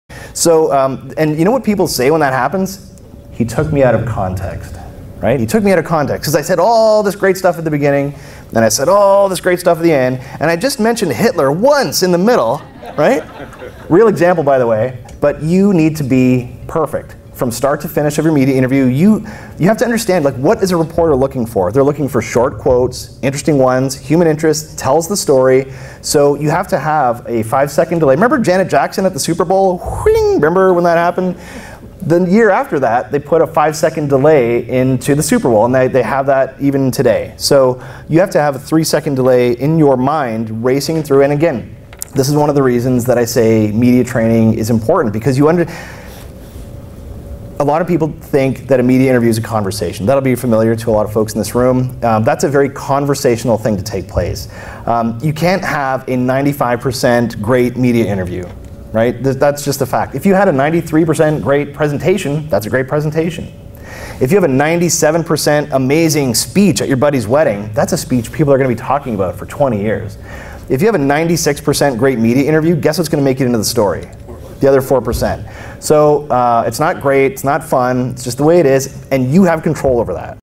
What can Janet Jackson teach you about media interviews?
Remember Janet Jackson's infamous wardrobe malfunction at the Superbowl? The NFL instituted a change to the half-time show after that incident - a change that you can use to your benefit in your media interviews...